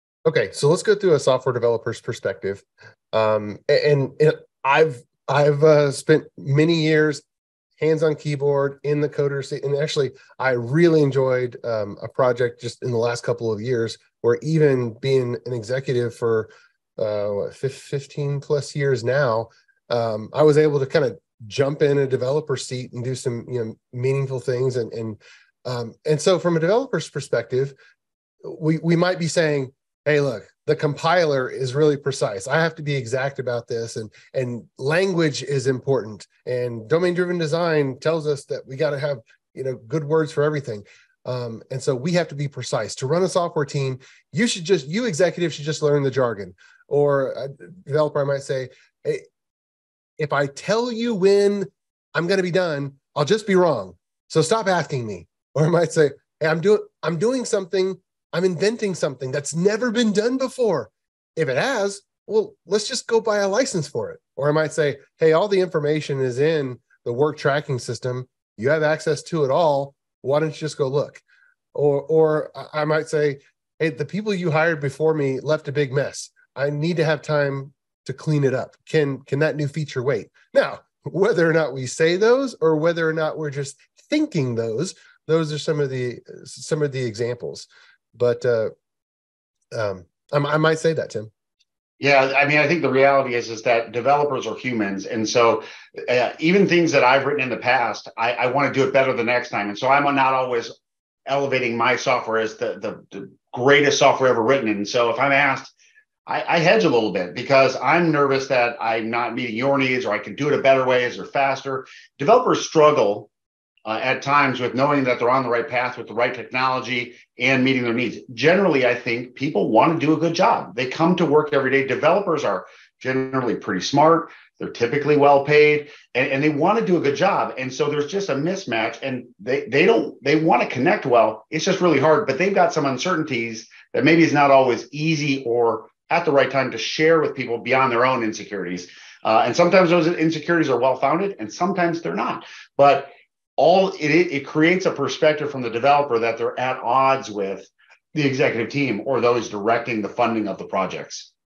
Software Developer Perspective | Webinar Clips, Executives & Developers Miscommunication
Schedule a Free 1 on 1 session with a Chief Architect

“Executives don’t understand what we do!” exclaims the developer. “Developers don’t understand how much this is costing us!” the executive responds.

Across our industry, executives are frustrated that they can’t get straight answers to simple questions like “When will it be finished?” And Developers are frustrated that executives are always changing their minds, asking for ambiguous things, and then wanting concrete deadlines.

In this webinar, we are going to explore the reasons for the communication mismatch. We'll share how developers can better speak the executive language and how executives can better communicate with their software teams.

The webinar host, Jeffrey Palermo, has been a programmer since 1997 and has been managing programmers since 2006. He has bridged both worlds, loves helping software teams succeed, and enjoys helping executives oversee their software teams better.